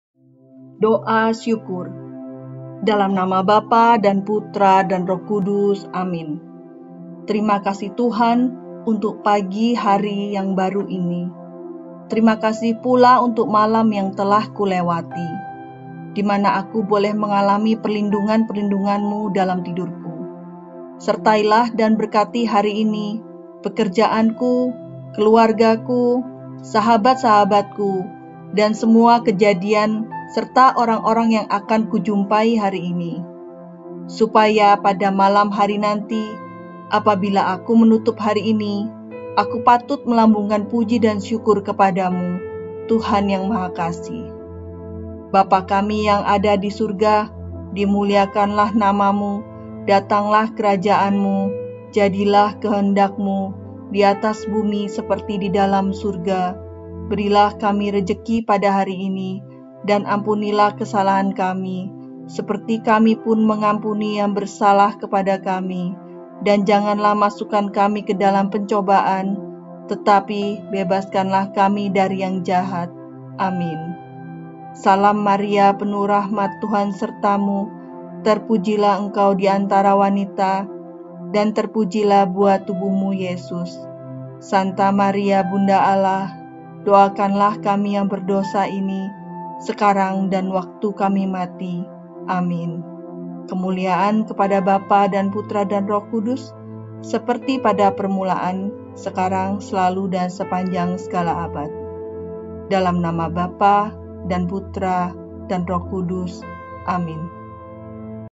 Doa Syukur Pagi Katolik | Mengucap Syukur, Mohon Kelancaran Rezeki dan Keselamatan
Doa Pagi Katolik Mengucap Syukur - Mohon Kelancaran Rejeki dan Keselamatan
"Terima kasih Tuhan, untuk pagi dan hari yang baru ini. Terima kasih pula untuk malam yang telah kulewati. Dimana aku boleh mengalami perlindungan-perlindungan-Mu dalam tidurku. Sertailah dan berkatilah hari ini, pekerjaanku, keluargaku, sahabat-sahabatku dan semua kejadian serta orang yang akan kujumpai hari ini. Supaya pada malam hari nanti apabila aku menutup hari ini, aku patut melambungkan puji dan syukur kepada-Mu, Tuhan yang Maha kasih.

Pagi hari, sebelum memulai aktivitas harian, kita sujud kepada Tuhan, untuk mengucap syukur atas kehidupan yang masih bisa kita nikmati hingga hari ini. Kita juga mohon berkat dan kelancaran rejeki untuk hari ini, serta keselamatan. Semoga hari ini kita dijauhkan dari segala malapetaka, sakit penyakit, dan kematian mendadak, serta segala bentuk kemalangan. Semoga hari ini Tuhan juga melindungi kita dari segala upaya jahat, ketidakadilan, dan kecurangan, yang mungkin hendak dilakukan orang lain terhadap kita.

Semoga dengan bantuan Roh Kudus, kita dimampukan untuk taat dan setia hanya pada kehendak-Nya. Amin.

Semoga semua doa kita terkabul.
AMIN